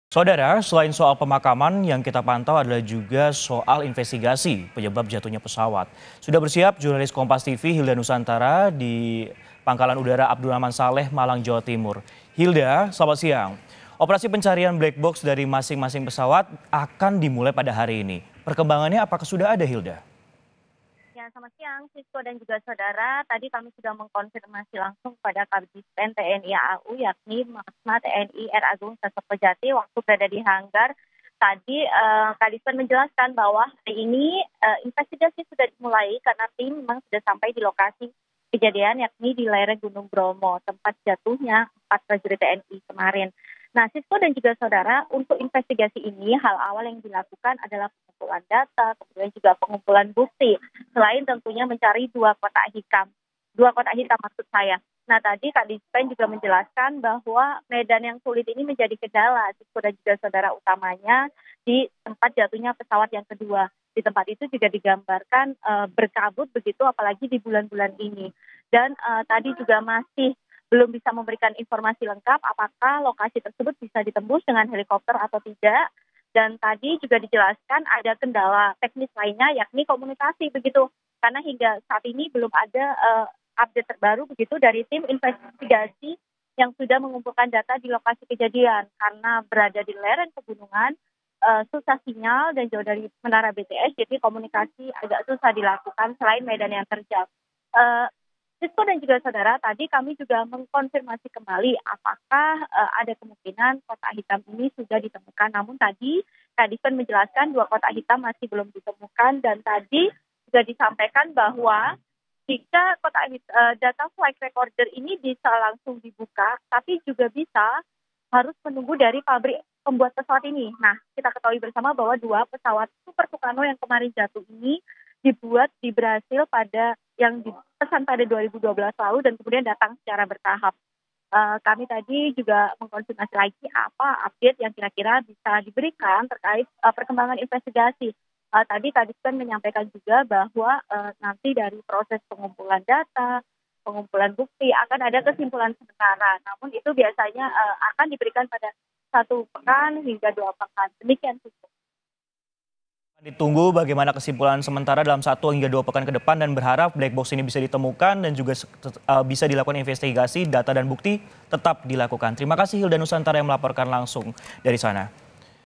Tim Investigasi Cari Dua Black Box Pesawat TNI AU yang Jatuh di Pasuruan
MALANG, KOMPAS.TV - Dugaan sementara jatuhnya dua pesawat TNI AU di Lereng Gunung Bromo, Pasuruan diakibatkan cuaca buruk.

Diduga pilot tak bisa melihat saat posisi pesawat sudah berada di dekat Lereng Gunung.

Kadispen TNI AU Marsekal Pertama TNI Agung Sasongkojati memastikan kedua pesawat yang dibeli dari Brasil pada 2012 lalu itu dalam kondisi baik dan layak terbang.

Sementara itu, hari ini (17/11/23) tim akan mencari dua kotak hitam pesawat TNI AU. Seperti apa perkembangannya, berikut informasi langsung dari jurnalis KompasTV Hilda Nusantara di Lanud Abdulrachman Saleh, Malang, Jawa Timur.